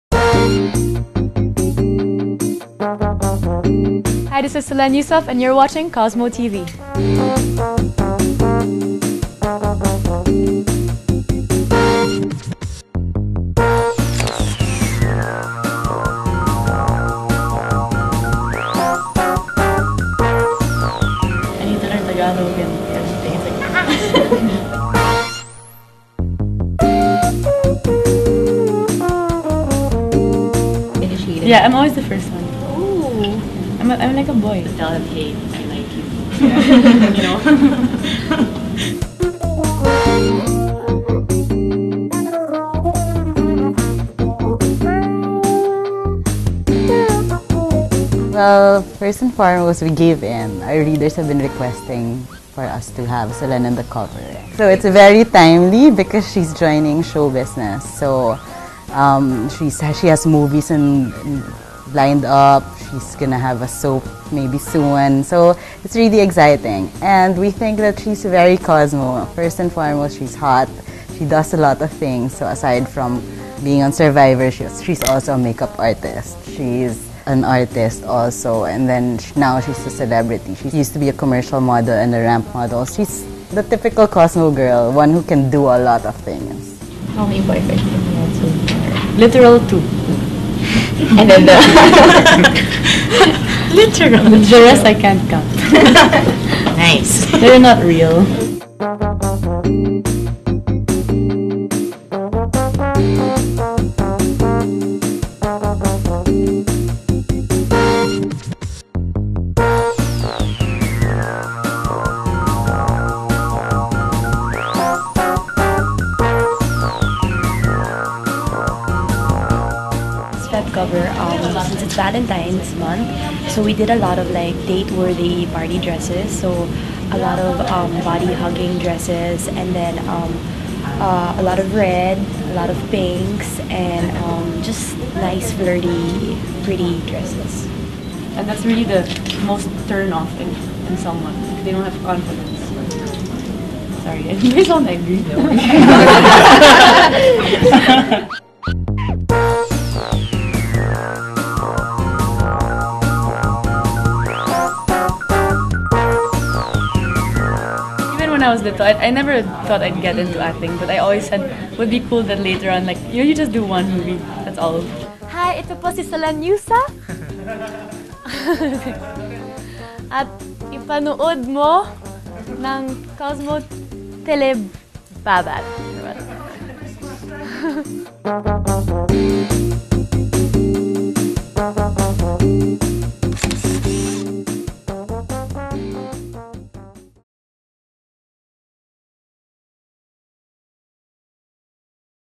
Behind The Scenes: Solenn Heussaff February 2011 Cover Shoot
Do you know that Solenn is a big clown? She just loves to goof around! Watch behind-the-scenes footage of our photo shoot with Solenn to see what we mean